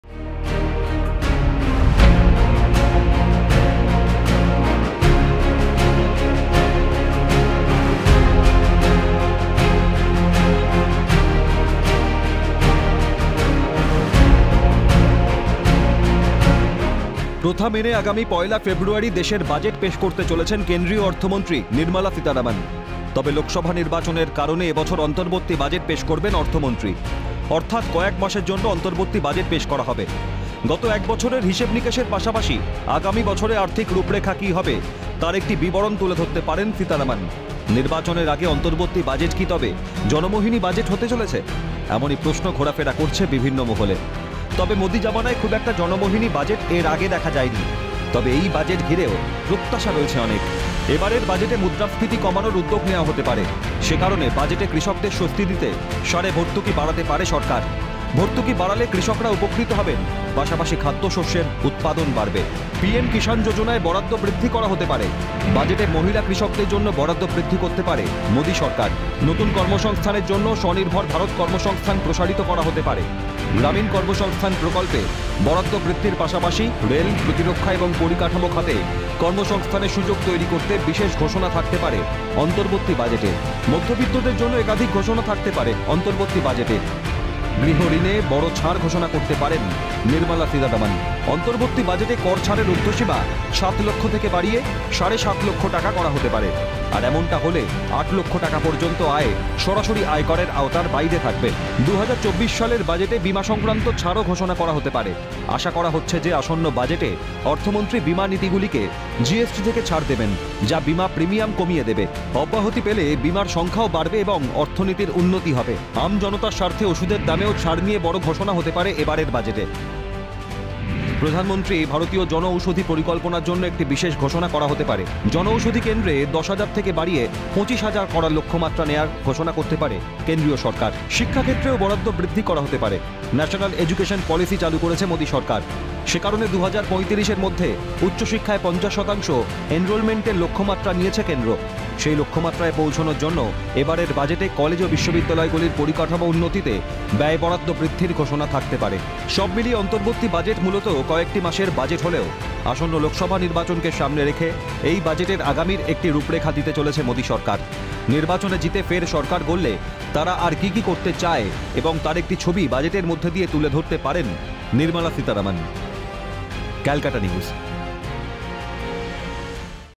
প্রথা মেনে আগামী ১ ফেব্রুয়ারি দেশের বাজেট পেশ করবেন কেন্দ্রীয় অর্থমন্ত্রী নির্মলা সীতারমন
প্রথা মেনে আগামী ১ ফেব্রুয়ারি দেশের বাজেট পেশ করবেন কেন্দ্রীয় অর্থমন্ত্রী নির্মলা সীতারমন  | ক্যালকাটা নিউজ ডিজিটাল

প্রথা মেনে আগামী ১ ফেব্রুয়ারি দেশের বাজেট পেশ করবেন কেন্দ্রীয় অর্থমন্ত্রী নির্মলা সীতারমন। লোকসভা নির্বাচনের কারণে এবছর অন্তর্বর্তী বাজেট পেশ করবেন অর্থমন্ত্রী। তবে এবছরের বাজেট ঘিরেও প্রত্যাশা রয়েছে অনেক।